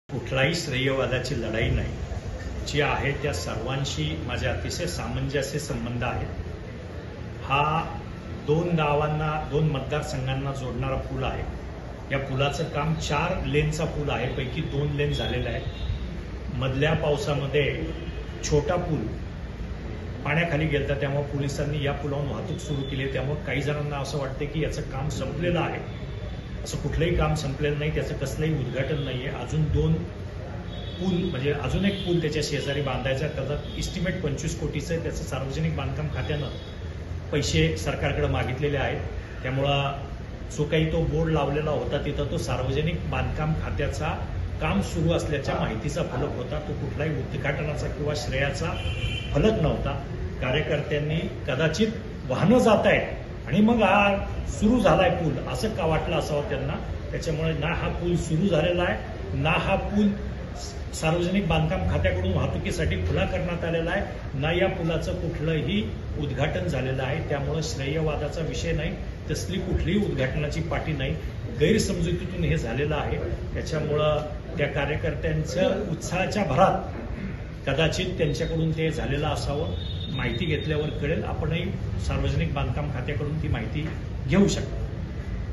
मांजरी उड्डाणपुलाचा नामफलक,काय म्हणाले Chetan Tupe?।Maharashtra Politics।
भाजपच्या आमदारांनी केलेल्या कामाचं श्रेय लाटण्याच्या प्रयत्न करणाऱ्या हडपसरचे राष्ट्रवादी काँग्रेसचे आमदार चेतन तुपे यांच्या कार्यपद्धतीवर नाराजी व्यक्त करत भाजप पदाधिकाऱ्यांनी मांजरी उड्डाणपुलाच्या नामफलकास काळे फासून आंदोलन केलं. यावर आता आमदार चेतन तुपे यांनी प्रतिक्रिया दिली आहे.

NS_OP_0924